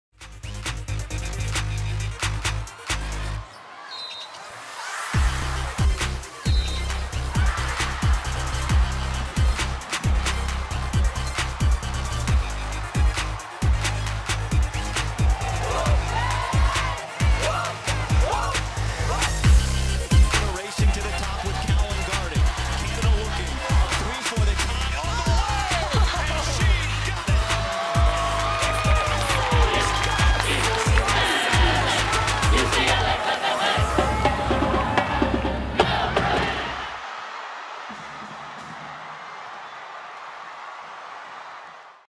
Made In March NCAA Basketball Tournament Commercial
UCLA preps to head to the NCAA Tournament!